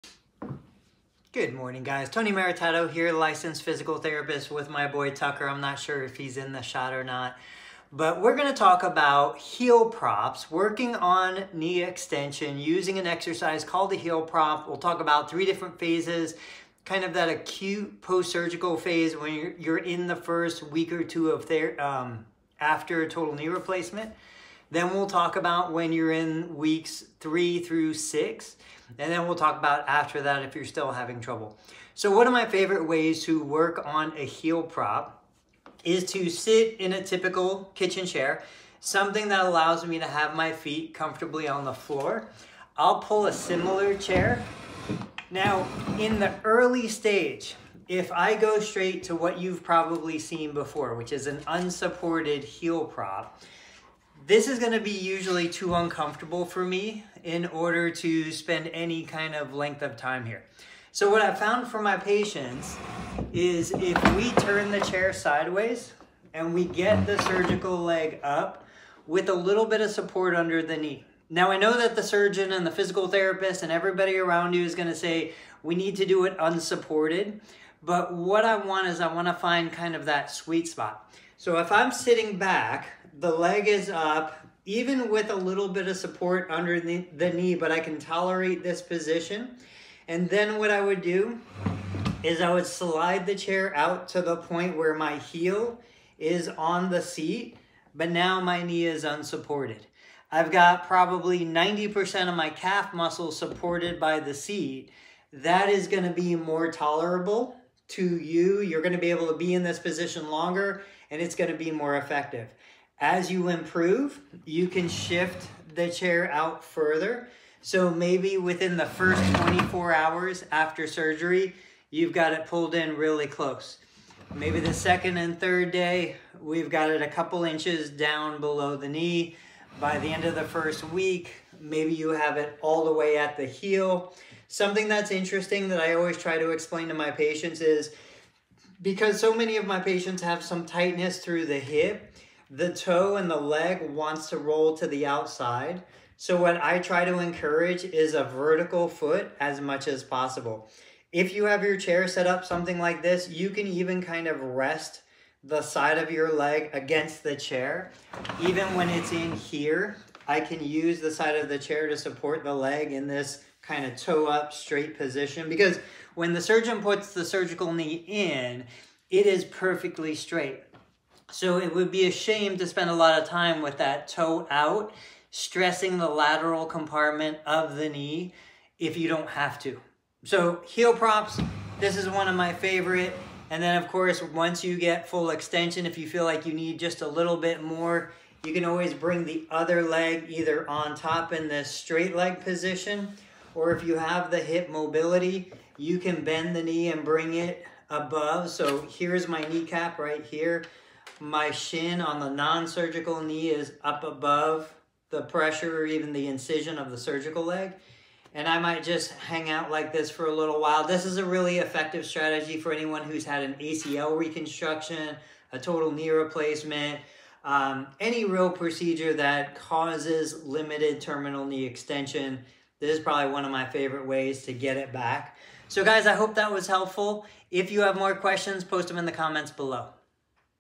Day 1 Exercises After Total Knee Replacements: Heel Props for Knee Extension
Heel Props are a great exercise that you can start performing during the first week after total knee replacement surgery. In this video, I show you how to make heel props more comfortable.